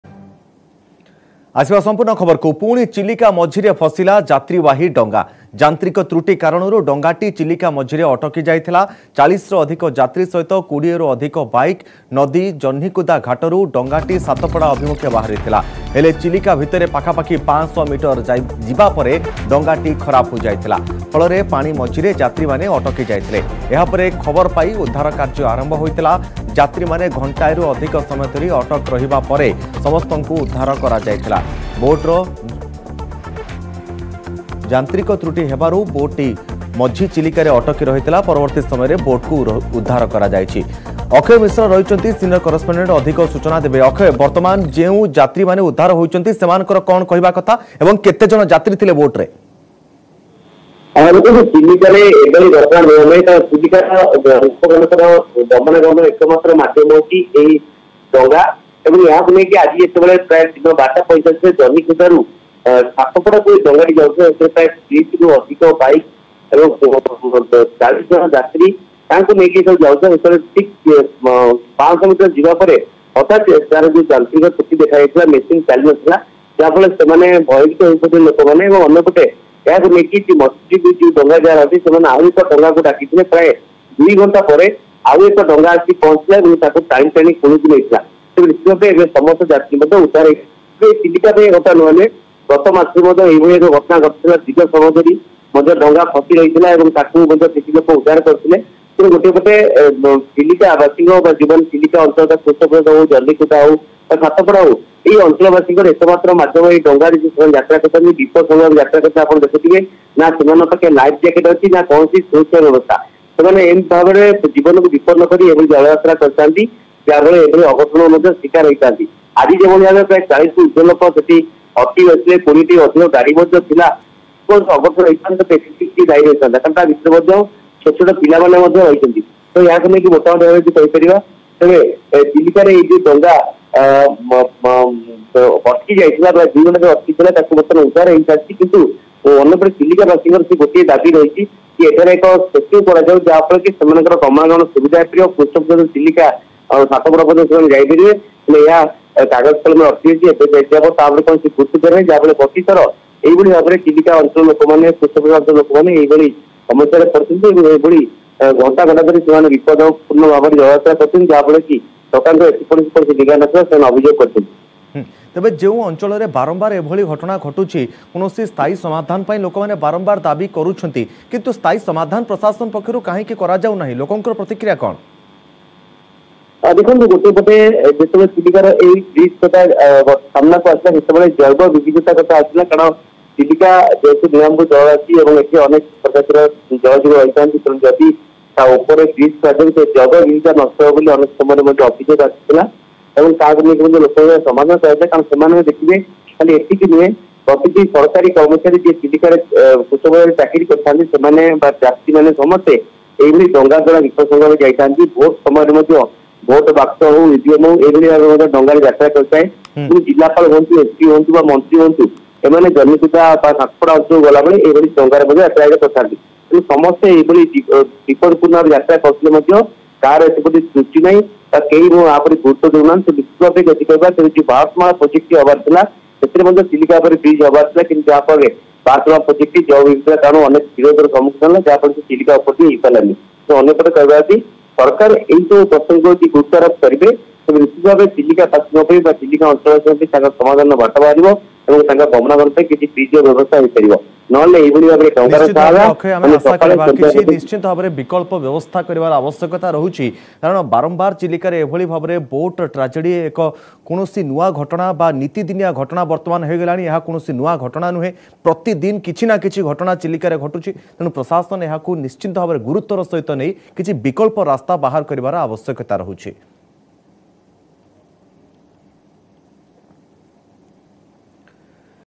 Chilika Boat Tragedy: ଚିଲିକା ମଝିରେ ଫସିଲା ଯାତ୍ରୀବାହୀ ବସ
ଚିଲିକା ମଝିରେ ଫସିଲା  ଯାତ୍ରୀବାହୀ ବସ | ଯାନ୍ତ୍ରିକ କାରଣରୁ ଫସିଲା ଯାତ୍ରୀବାହୀ ଡଙ୍ଗା | ଡଙ୍ଗାରେ ଥିଲେ ୪୦ରୁ ଅଧିକ ଯାତ୍ରୀ |  Janhikuda ଘାଟୀରୁ ସାତପଡ଼ା ଯାଉଥିଲା ଡଙ୍ଗା |17thJune 2022 | News18 Odia